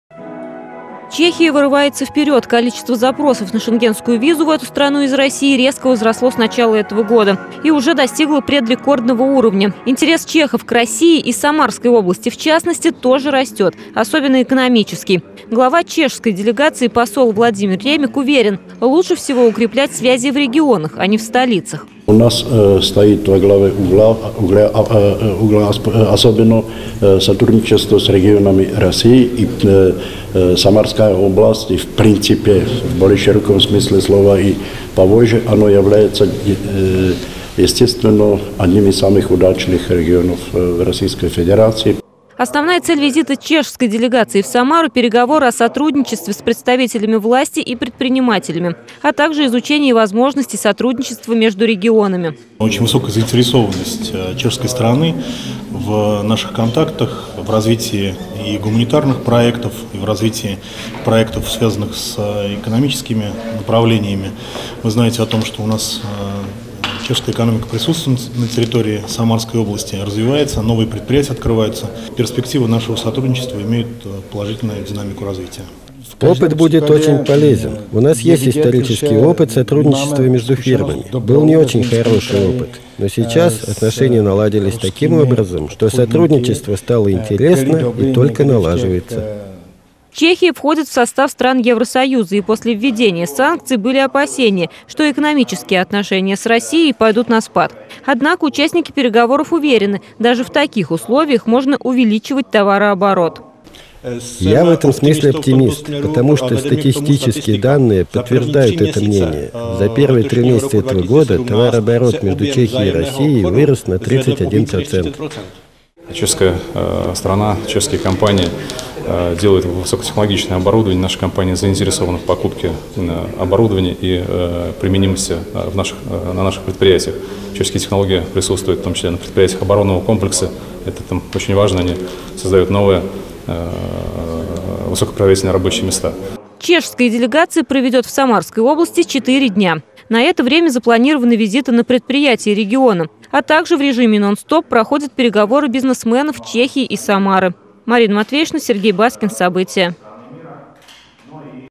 Прогноз на сотрудничество - оптимистичный. В Самару прибыла делегация из Чехии
Дружить регионами. В Самару прибыла делегация из Чехии. Представители органов власти и бизнес-сообщества обсудили возможности экономического сотрудничества. В самарской области уже представлены чешские предприятия - в частности завод по производству свечей зажигания. Сейчас участники деловых переговоров ищут новых партнеров и новые направления сотрудничества.